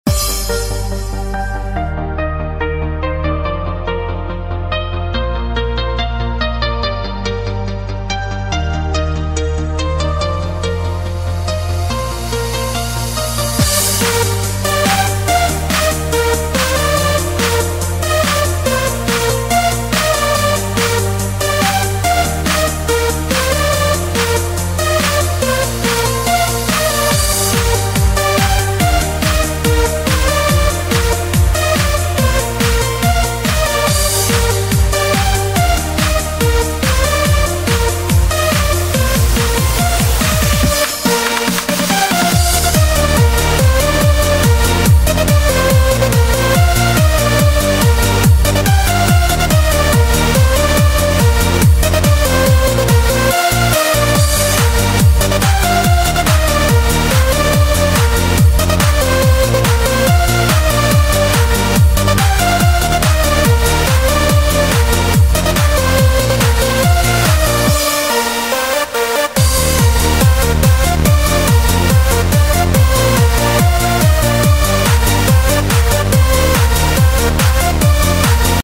[4K 60fps] Black And Yellow 2 by Nena Kiwi (3 "Coins")
Level Info:

Level Name: Black And Yellow 2
Level ID: 1777958
Level Author: Nena Kiwi
Level Difficulty: Hard 5★